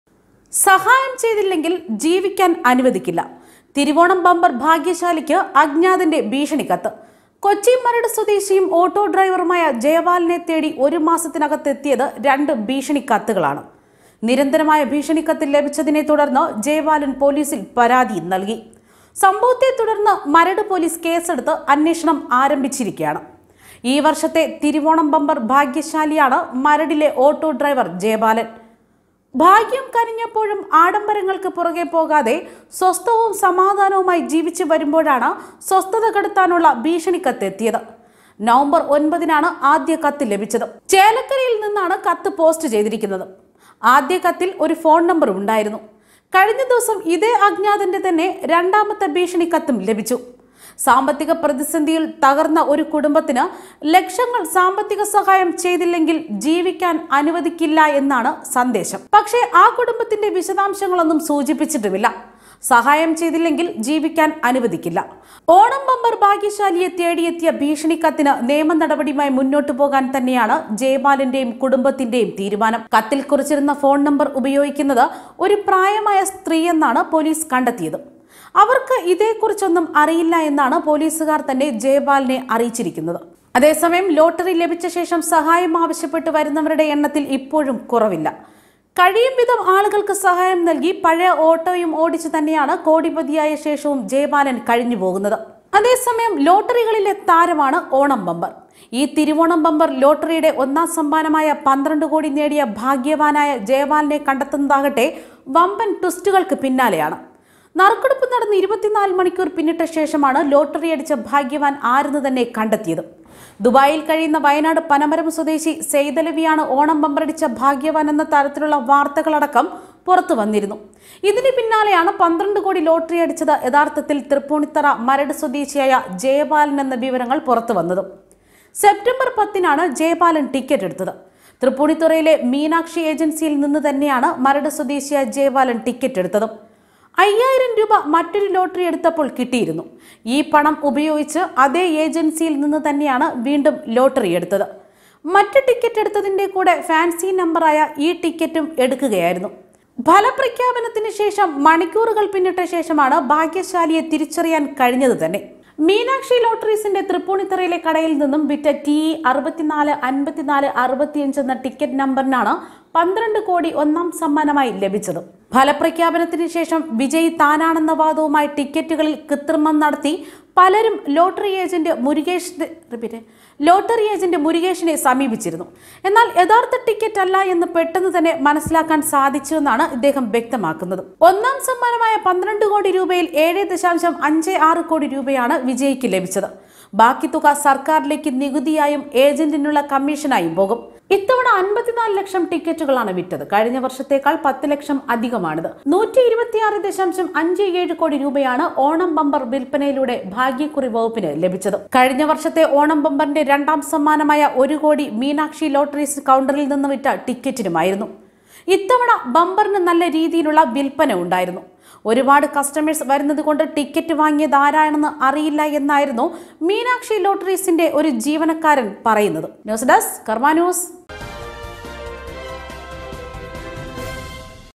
ഓണം ബംപര്‍ ഭാഗ്യശാലിയെ തേടി അ‍ജ്ഞാതന്റെ ഭീഷണി കത്ത്
സഹായം ചെയ്തില്ലെങ്കില്‍ ജീവിക്കാന്‍ അനുവദിക്കില്ല,തിരുവോണം ബംപര്‍ ഭാഗ്യശാലിക്ക്  അ‍ജ്ഞാതന്റെ ഭീഷണി കത്ത് വീണ്ടും.